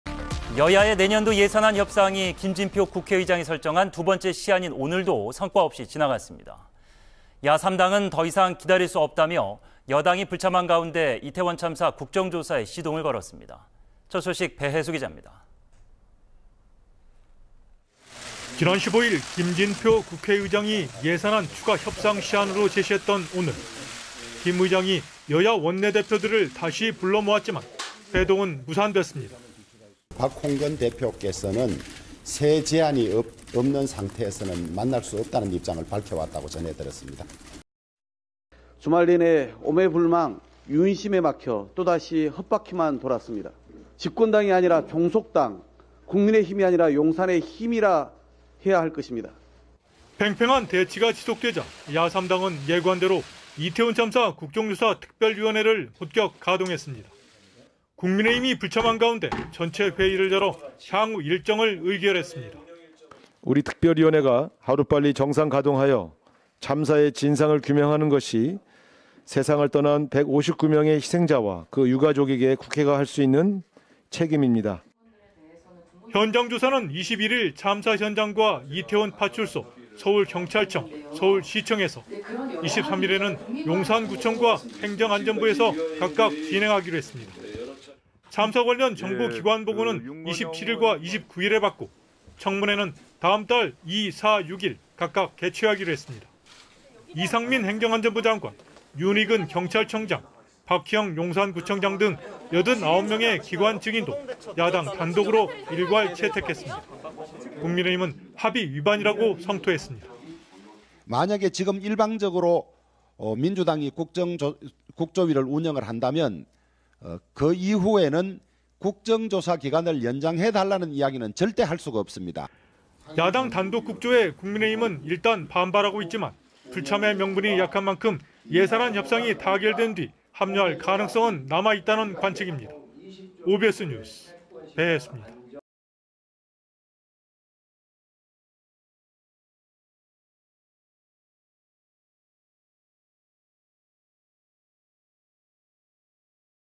"국민의힘 아닌 용산의힘"... 여야 예산안 처리 또 불발
여야의 내년도 예산안 협상이
김진표 국회의장이 설정한
두 번째 시한인 오늘도
성과 없이 지나갔습니다.
야 3당은 더 이상 기다릴 수 없다며
여당이 불참한 가운데,
이태원 참사
국정조사에 시동을 걸었습니다.
첫 소식, 배해수 기자입니다.